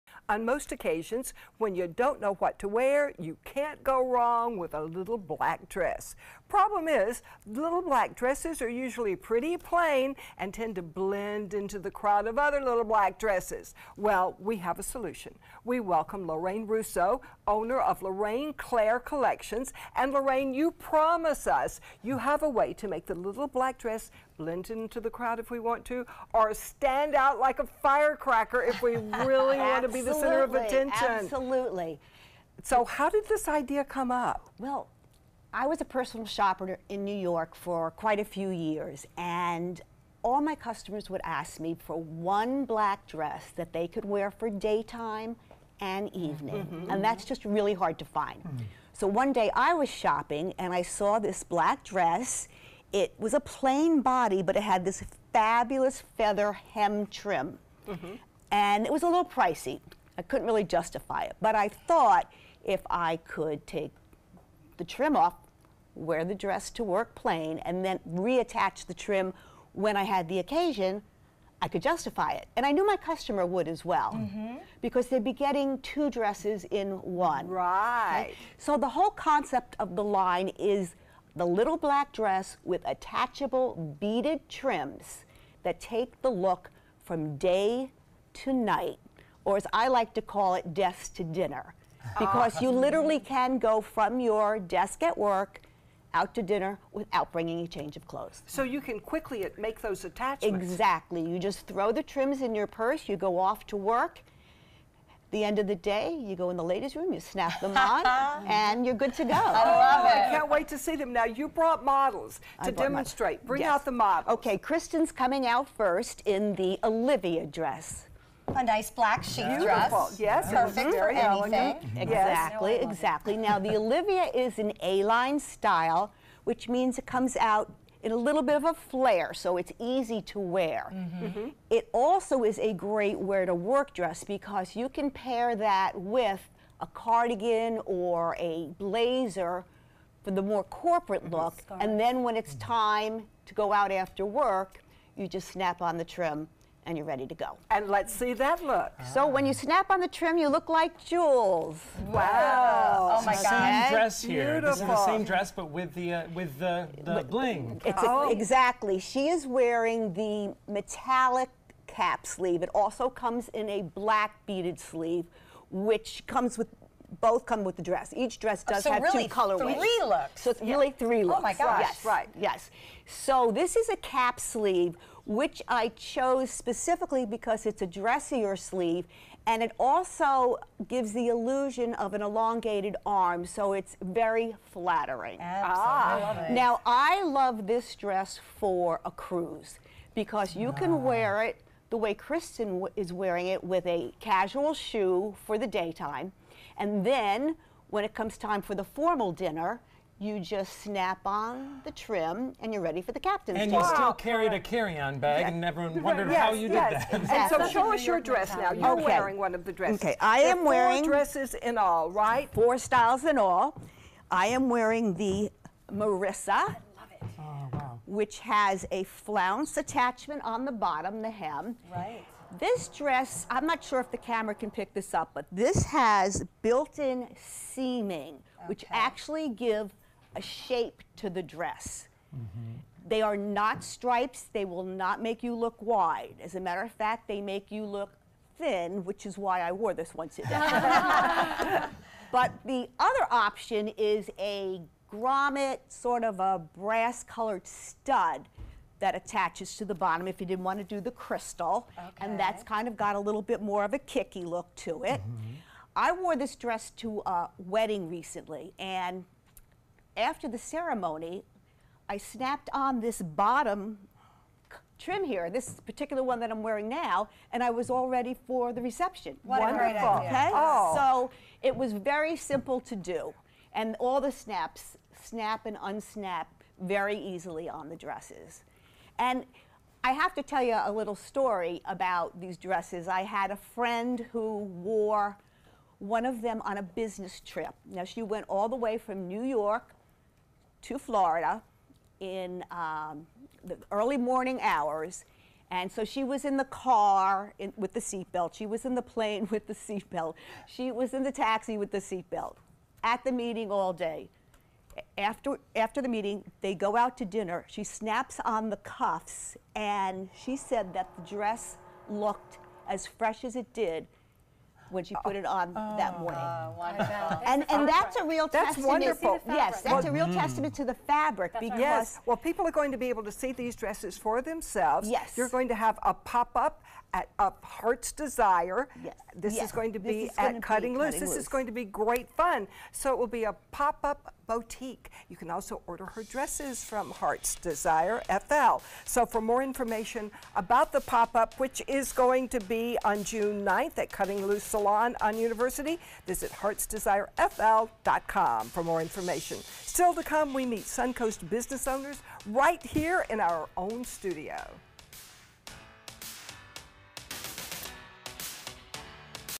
Lorraine Claire Dresses- Desk to Dinner
The little black dress with a twist-Spotlight on TV-
Lorraine Russo-Fashion Designer